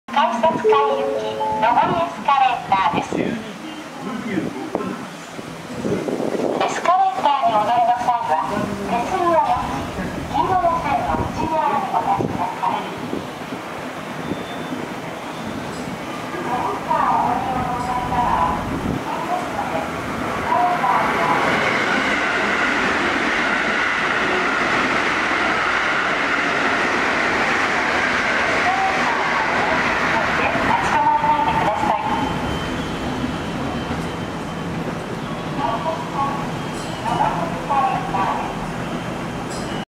【日立製】JR島本駅ホーム 昇りエスカレーター
JR島本駅ホームのエスカレーターです。